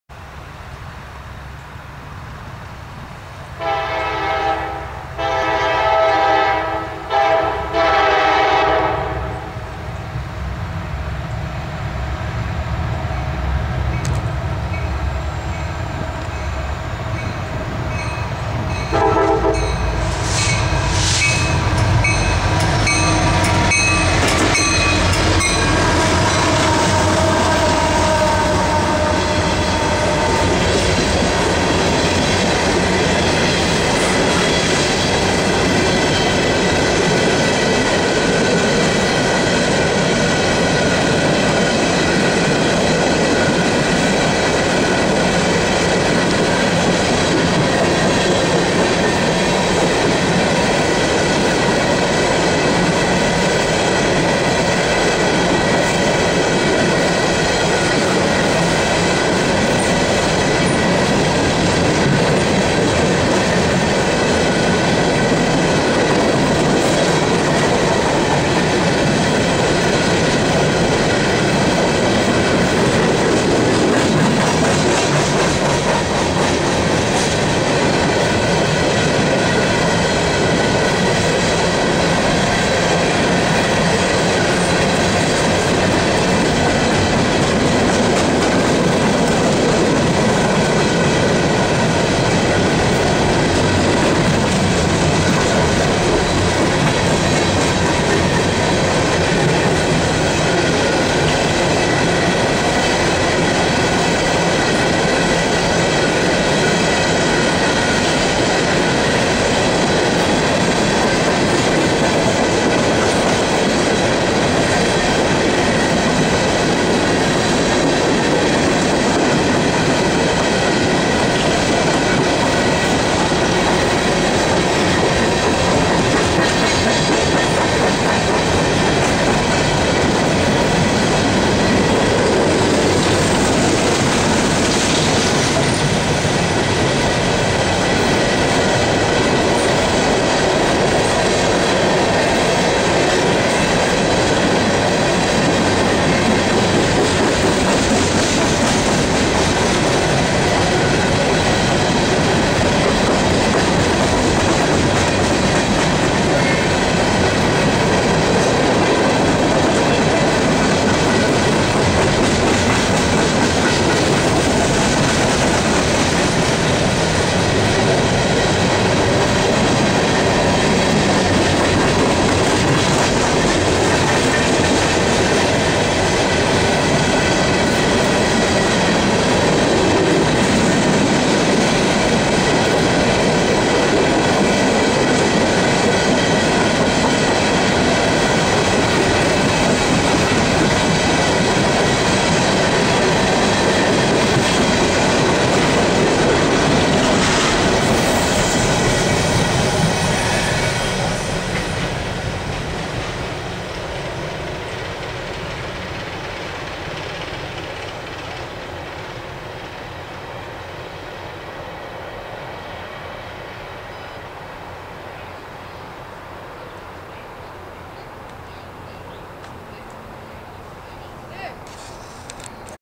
CSX EMPTY COAL DRAG PULLED BY SD70 4732 AN CW60 656
WOW SD70 AND CW60 TOGETHER